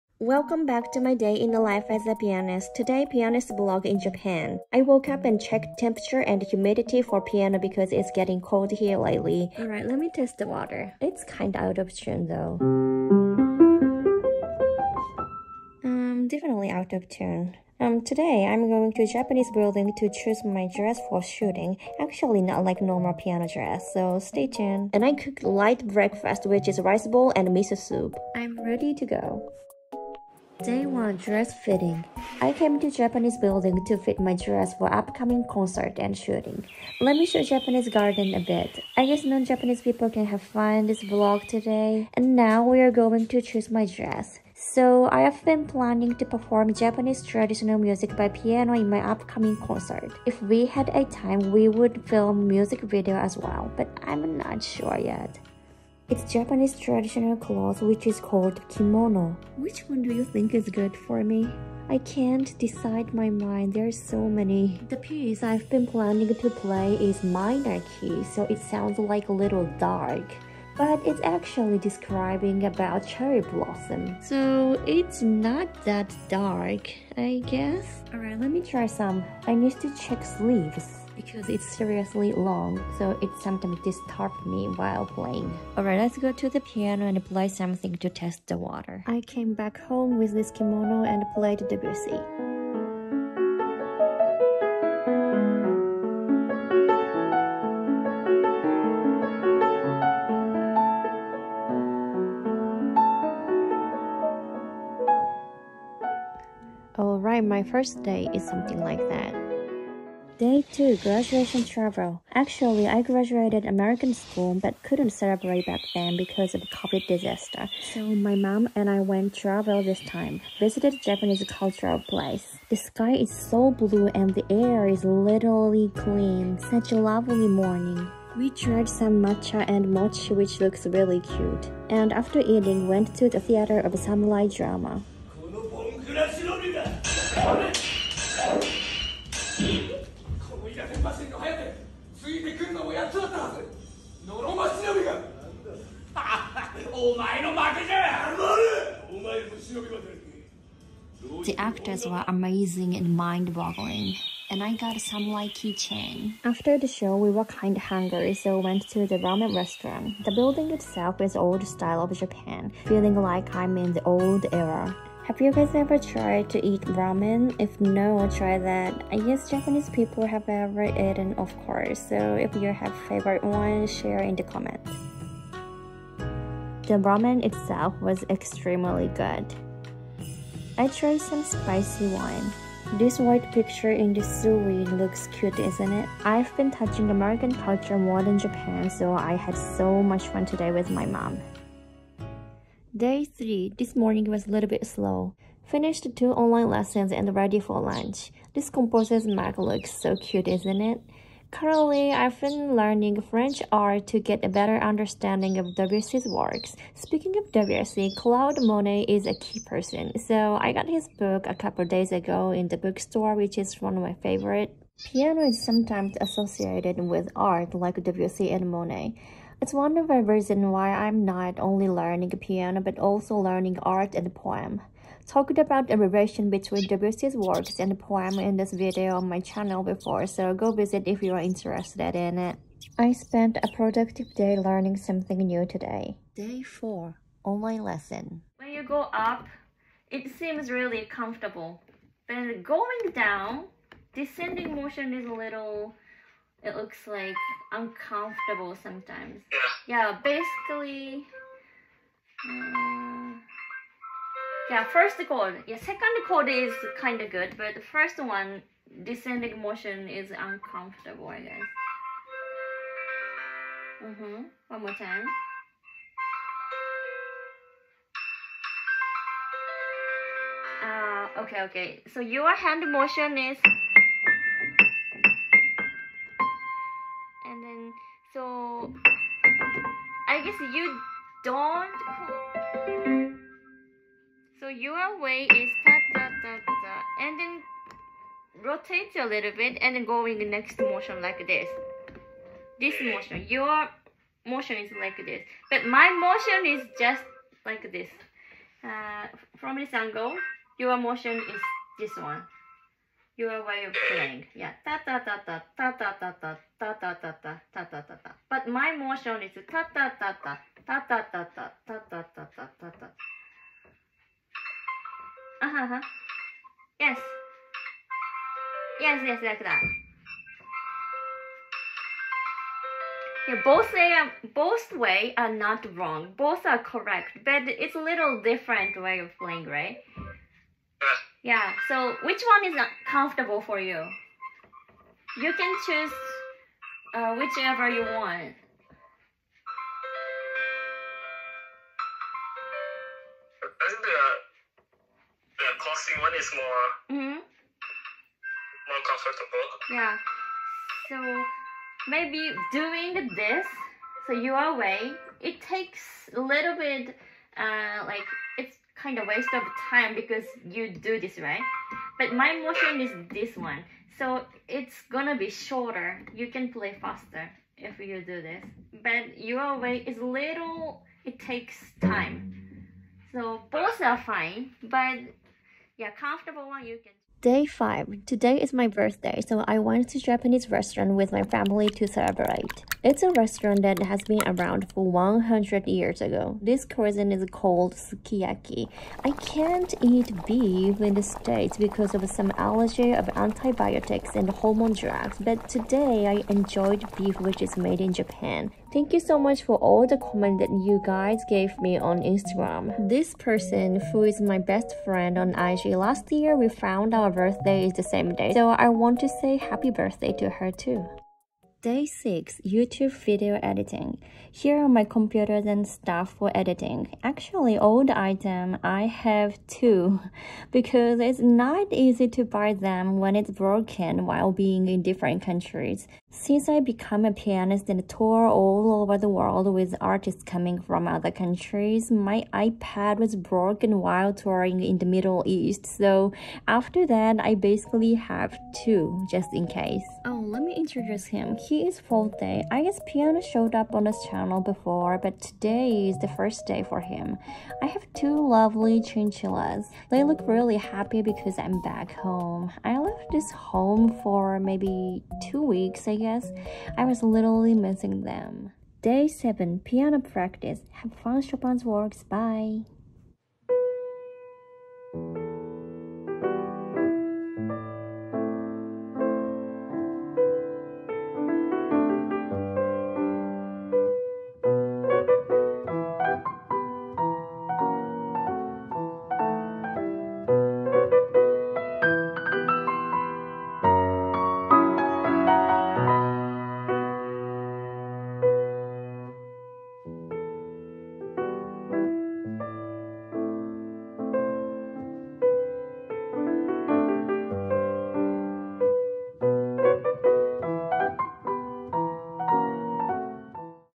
Pianist's 7 days vlog
Guys!! Japan piano vlog today!!

Hope you guys have fun this beautiful country with me :D